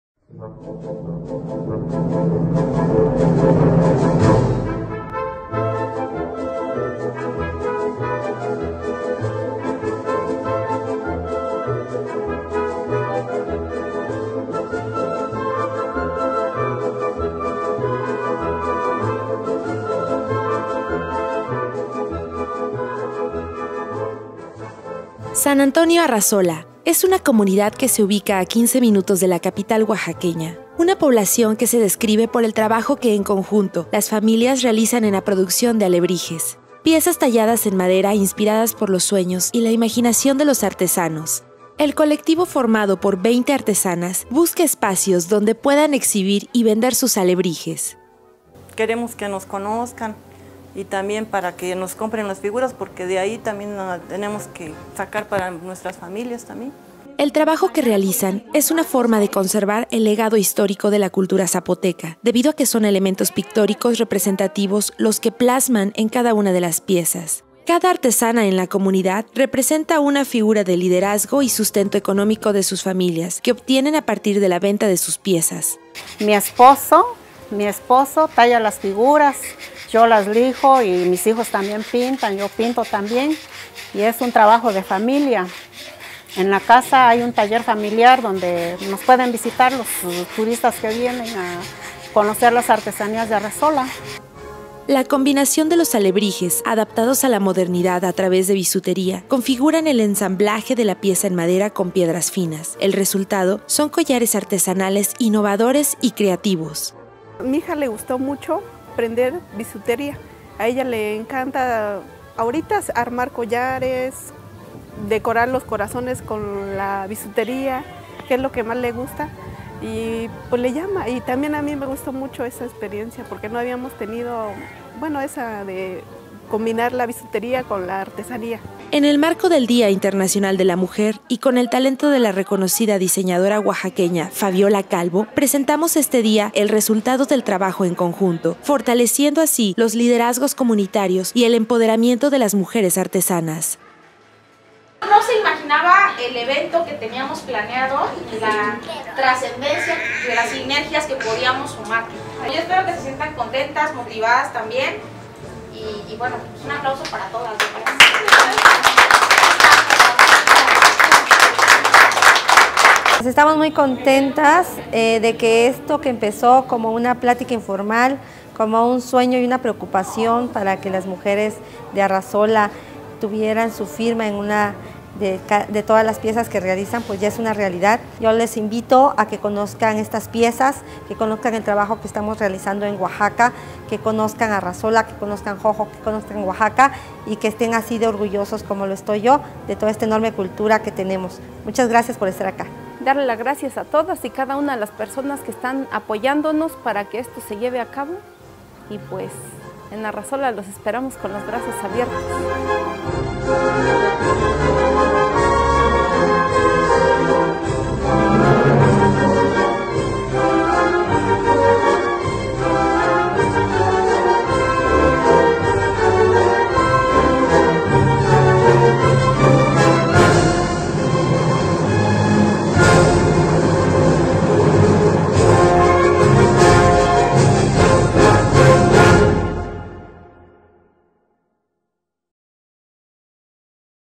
Pintando Sueños: Joyería Artesanal de Oaxaqueñas
Video de introducción para la exposición Pintando Sueños. Una muestra de joyería artesanal que manifiesta el empoderamiento femenino de las artesanas de Arrazola, Xoxocotlán en Oaxaca.